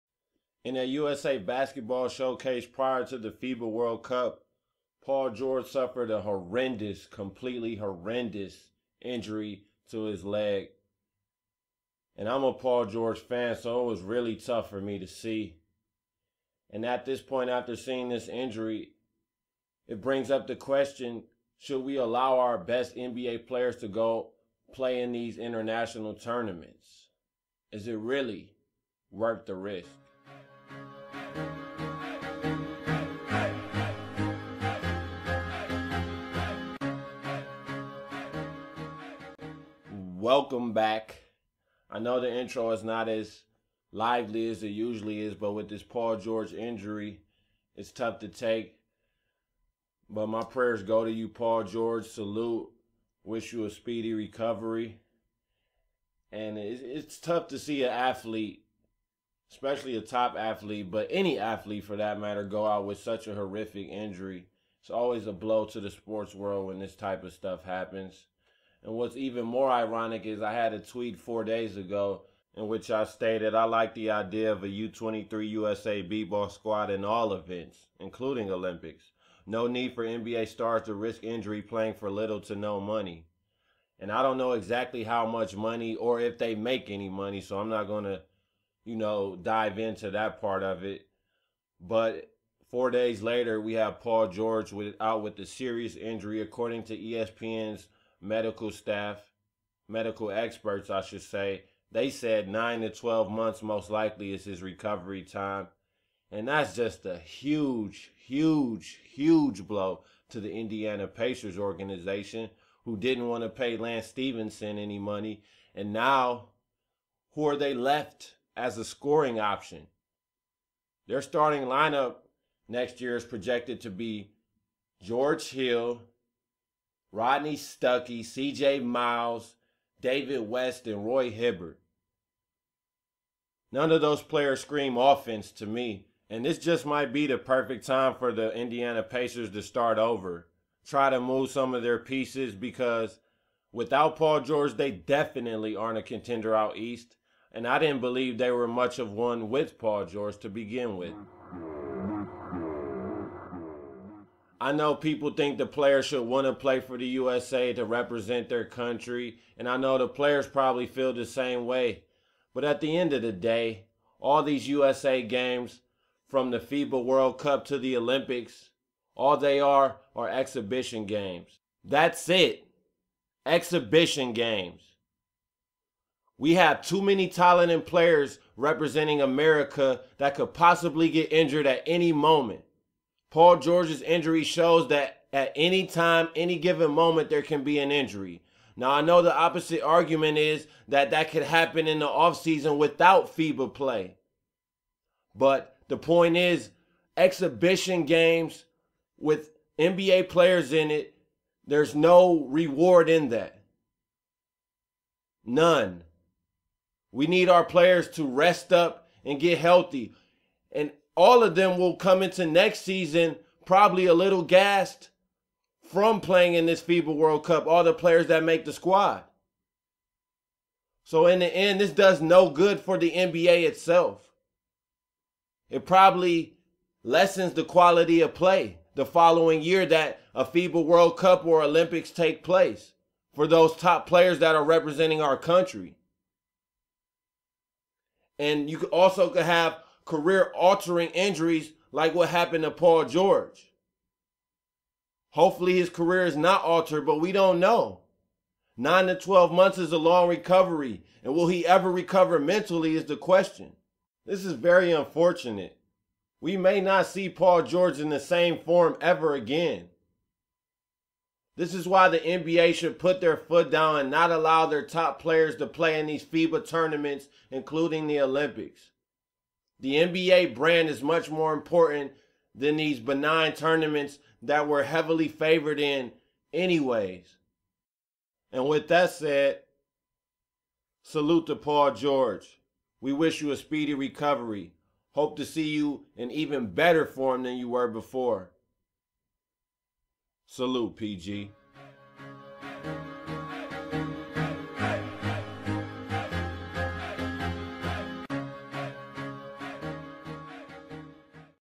Podcast: Paul George's horrific injury (broken leg) | Should NBA stars play for Team USA
Podcast consists of:

- Paul George's injury, what effect it has on the Pacers
- Will Paul George return the same
- Should NBA stars play for Team USA